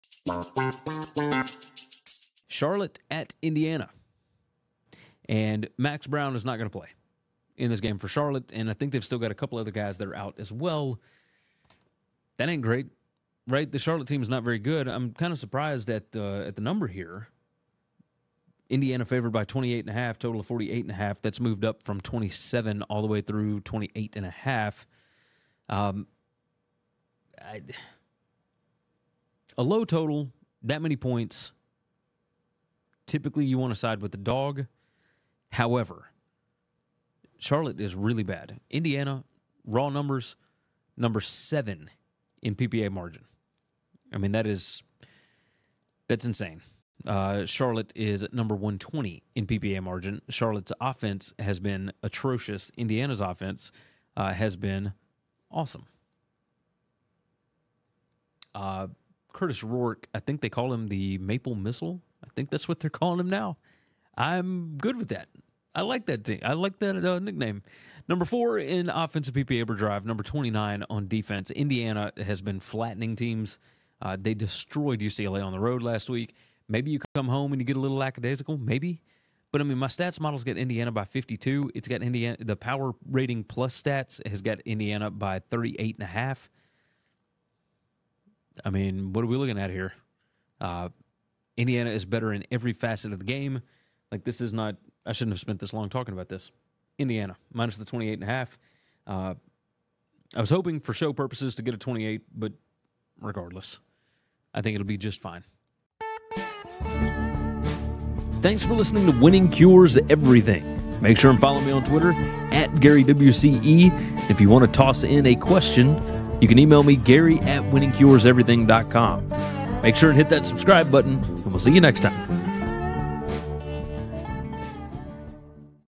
Indiana vs Charlotte Picks & Prediction Against the Spread 2024 College Football Analysis
Indiana vs Charlotte Picks Against the Spread 2024 Predictions

Gary gives a quick preview, analysis, score prediction based on advanced stats and data & his pick (lean) against the spread (ATS) for the 2024 College Football Week 4 matchup featuring Charlotte vs Indiana!